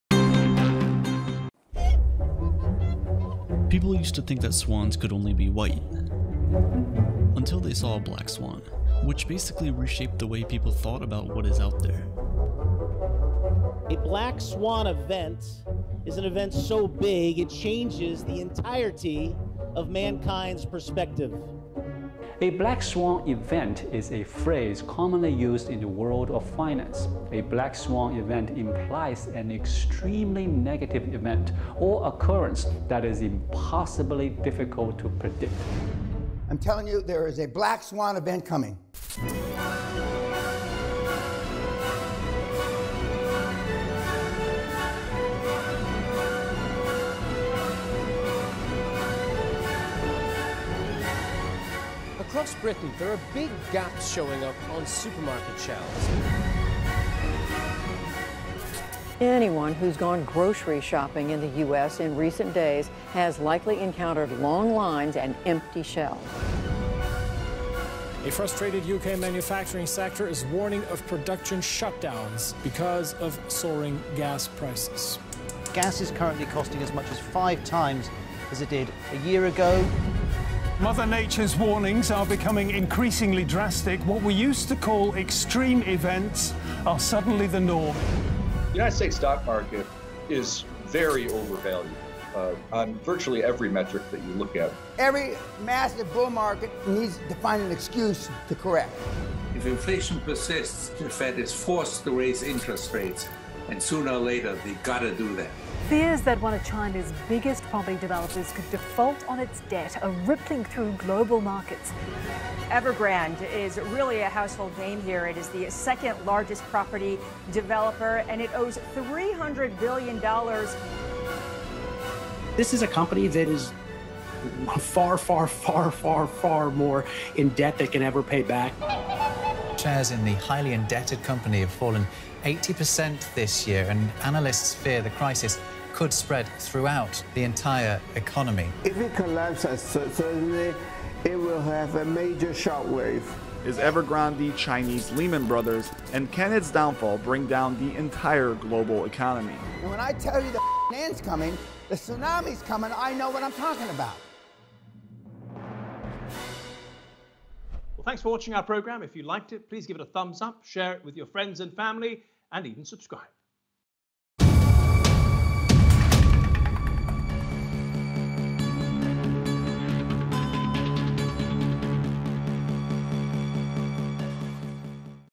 What is a 'black swan' event and is there one looming?
A 'black swan' event is a term used in the world of finance to describe unpredictable and catastrophic financial disruption. Warnings about an impending black swan event are growing louder. Could it be China's property market's about to implode? Or will it be hyperinflation, rising interest rates, or food shortages? Or all of the above.